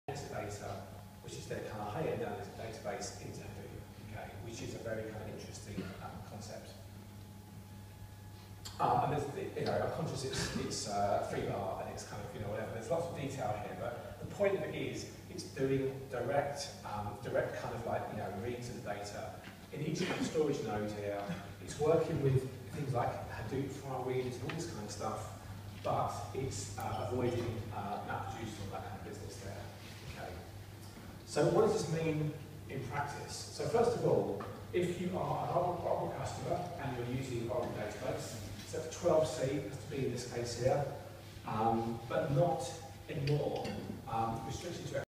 HUG Ireland - Short Event Footage 5 of 7 - 12th Sept 2016
Short footage 5 of 7 from a Hadoop User Group (HUG) Ireland event on Sept 12th in association with Oracle. Speakers were Mark Rittman and Jeff Richmond.